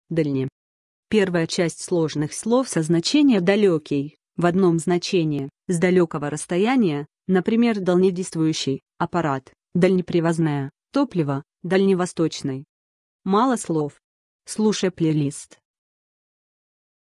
ДАЛЬНЕ... - что это такое? значение и описание
Хочешь знать что значит ДАЛЬНЕ...? тогда слушай до конца.

 Первая часть сложных слов со знач. далекий (в 1 знач.), с далекого расстояния, напр. далънедействующий (аппарат), дальнепривозное (топливо), дальневосточный.